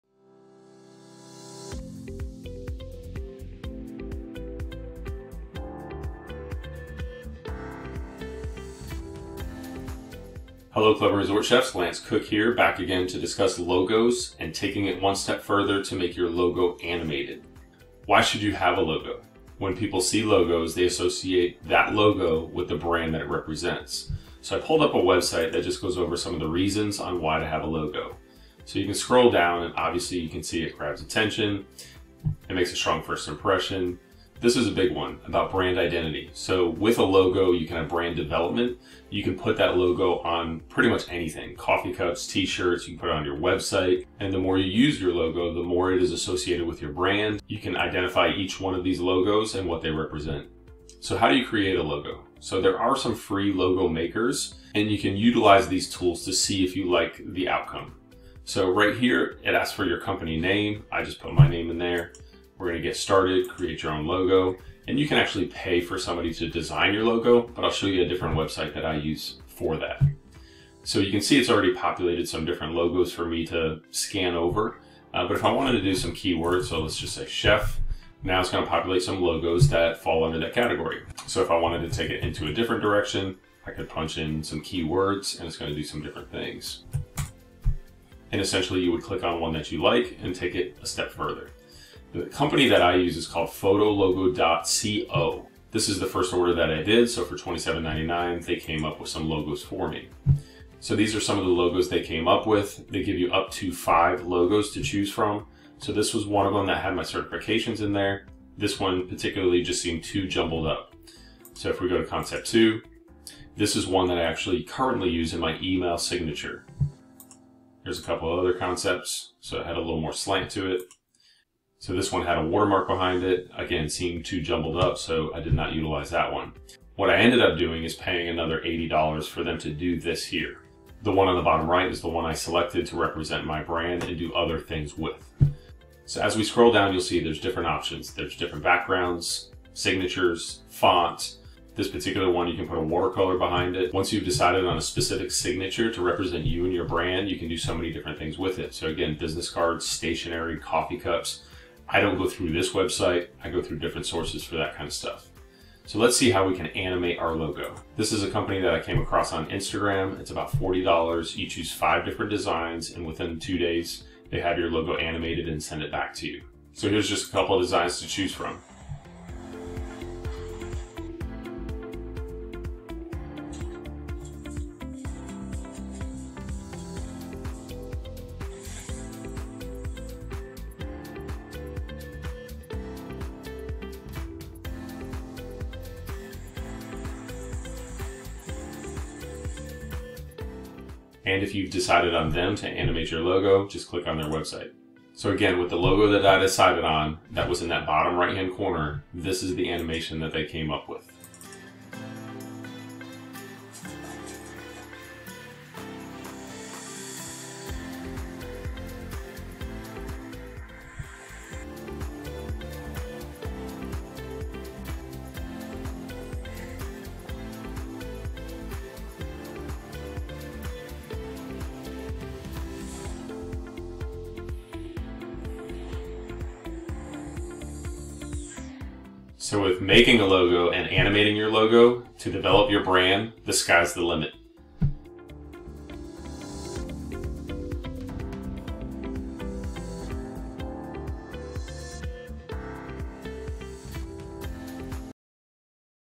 How to Create a Logo to Develop Your Brand Identity
In this video, released at the 2023 Chef to Chef Conference in Miami, Lance Cook, WCMC, MWMCS, CEC, CCA, CFBE, FMP, CFSM, Executive Chef of Hammock Dunes Club (Palm Coast, Fla.), explains his use of a personal logo to signify his expertise in the culinary industry. He details how other club chefs can create their own logo for personal brand recognition while highlighting the impact a logo can have on a chef's identity.